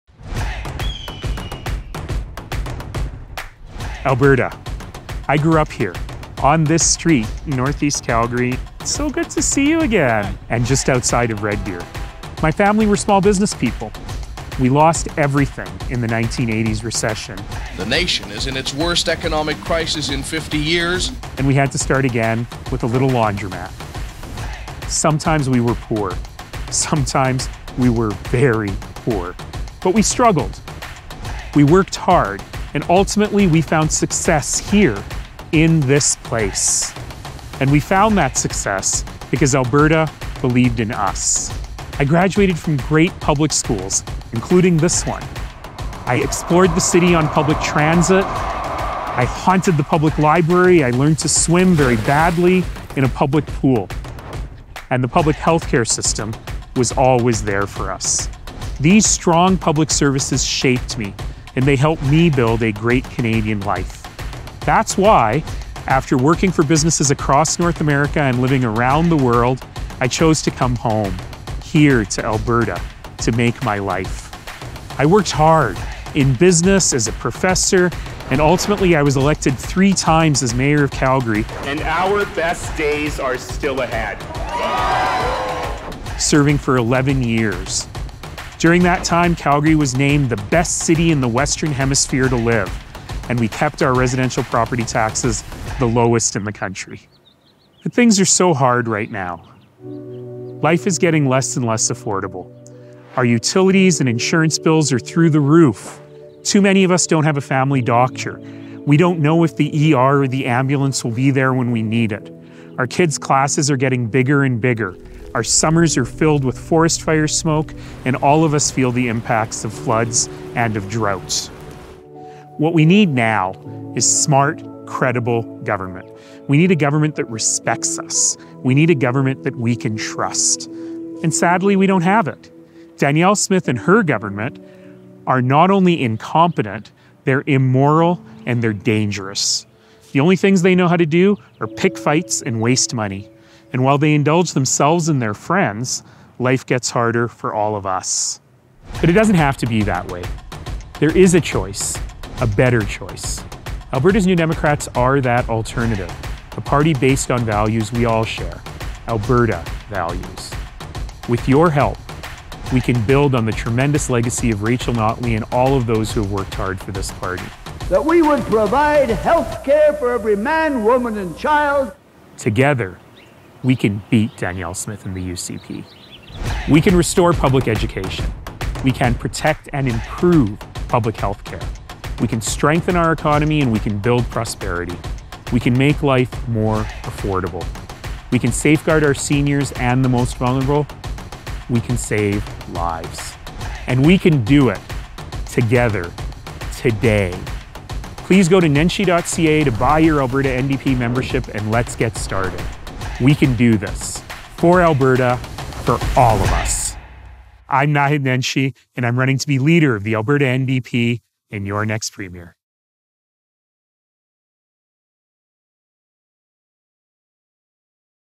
Naheed Nenshi for Alberta. For all of us.
I’m Naheed Nenshi. I grew up here, in Northeast Calgary and in Red Deer County. My family had good times and rough times, but we also always had community and strong public services when we needed them.

But today, we're facing crises on every front. Too few homes and what's there is unaffordable, while government-sanctioned utility and insurance hikes make life more expensive. Albertans are being asked to accept late ambulances and closed ERs, summers filled with wildfire smoke, no action to protect against flood and drought, and growing class sizes with under-resourced teachers. We watch entrepreneurs struggle with red tape and investment is leaving our province.

The UCP’s response is to distract the public and punish the weak and vulnerable because Danielle Smith only knows how to do two things: pick fights and waste money.

We can do better. Together.

That’s why I’m running to be the Leader of Alberta’s NDP and the next Premier of Alberta.

Together, we’ll continue the legacy of Rachel Notley and those who helped grow this party.

Together, we can deliver a smart, capable government that respects all Albertans.

I can’t do this without your support. Please fill out the form on this page and join the Alberta NDP so you can vote in the leadership race.

Let’s start today. For Alberta. For all of us.